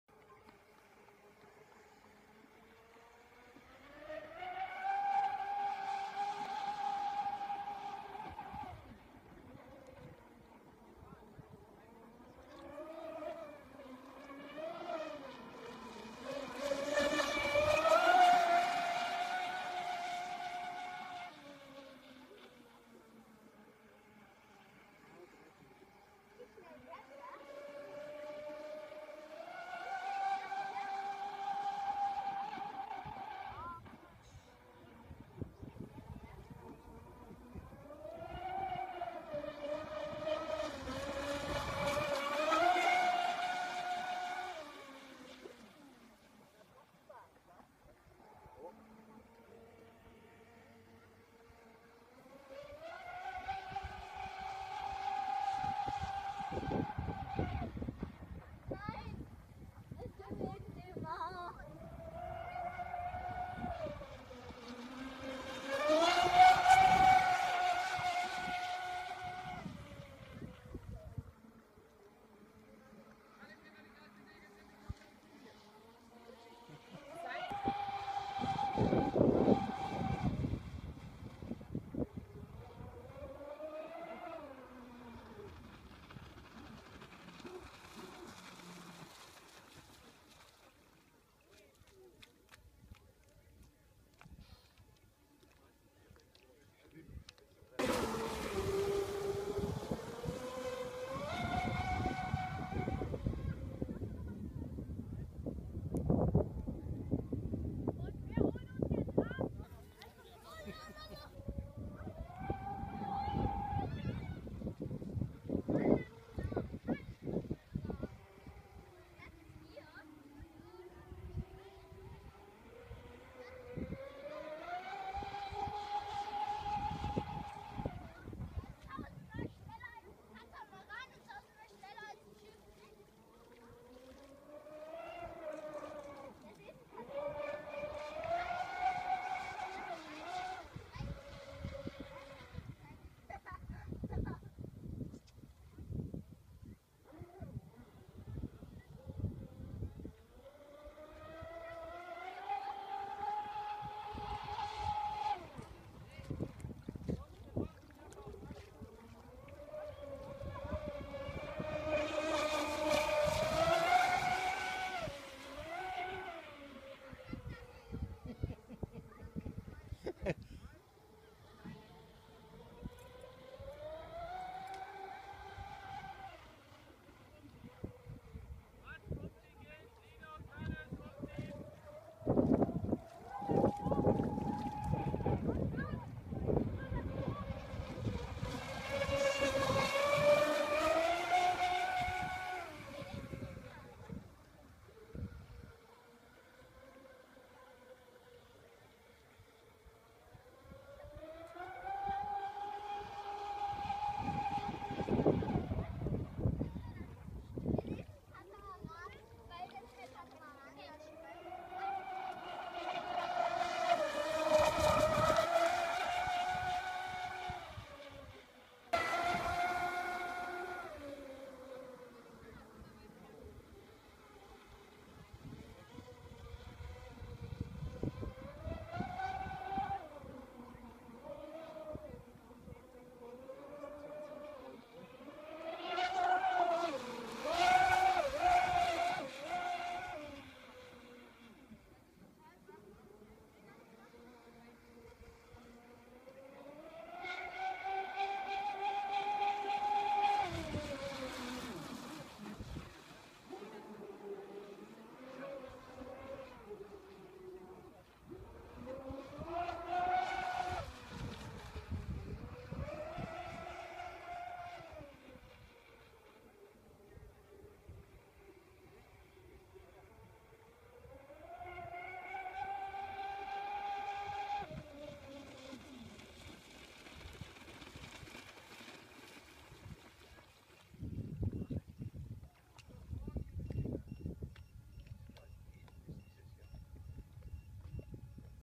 MHZ Ct03 auf Norderney
Länge über alles 177cm 15.5kg rtr Motoren TFL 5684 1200kv an 6s2P 21.6v pro Seite 10000milia bis zu 7min Fahrzeit Regler Seaking 180A